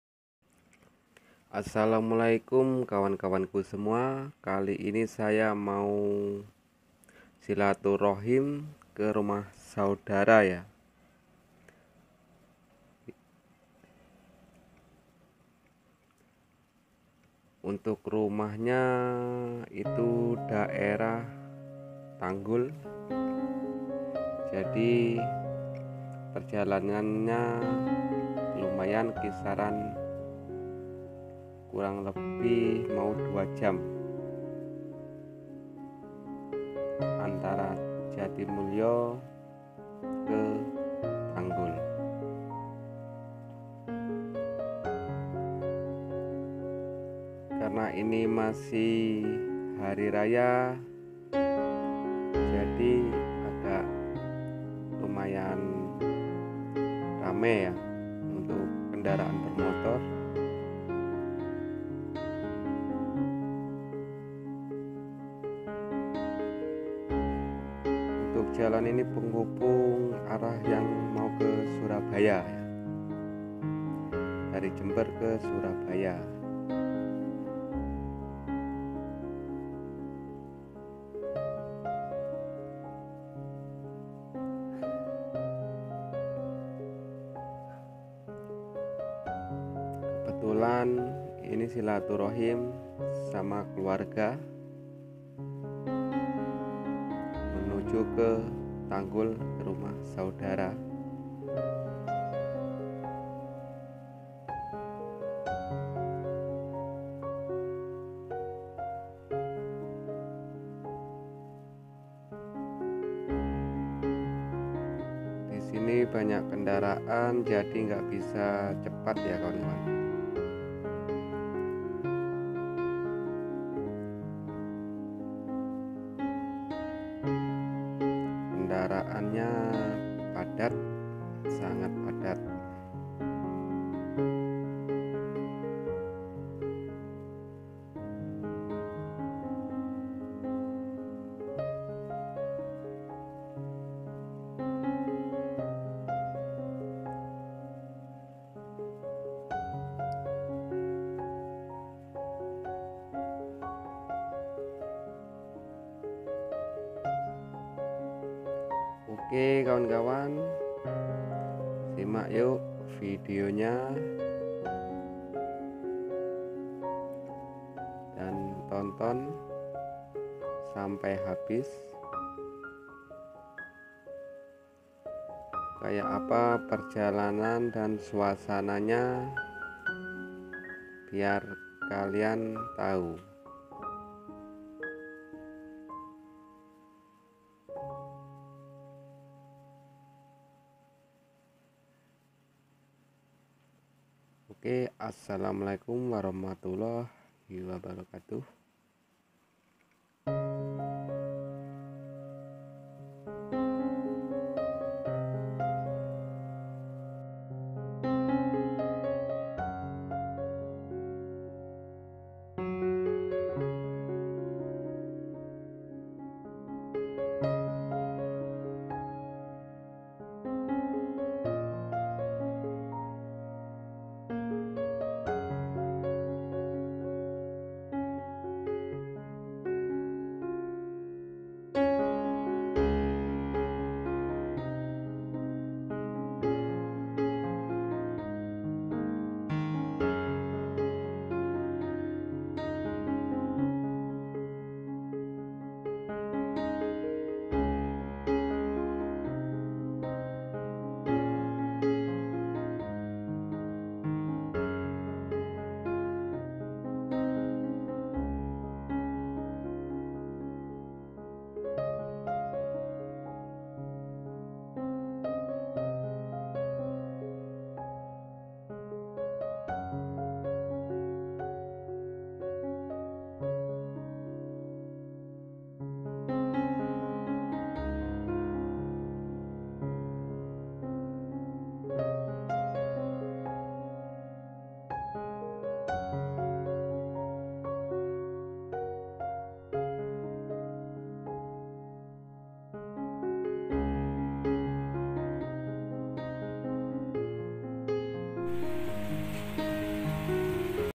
Silaturahmi Jatimulyo - Tanggol Rumah Saudara // ke Rumah Saudara Jember-Tanggol Pake mobil Calya
Assalamu'alaikum sobat..

Kali ini aku sama keluarga Silaturahmi ke Rumah Saudara, yang tepatnya di daerah Tanggal perjalanan dari rumah ke Tanggol kurang lebih ada 2 jam Kawan. antara laen melewati Jatimulyo Jenggawah Jember Tanggol Dalam perjalanan Seru banget nih kawan kawan..kayak apa serunya simak yok vidionya..

Silaturahmi Jatimulyo Jenggawah Jember Tanggol
Silaturahmi ke Rumah Saudara Tanggol Naik Mobil Toyota Calya

Gofila Cell Channel
Terimakasih kawan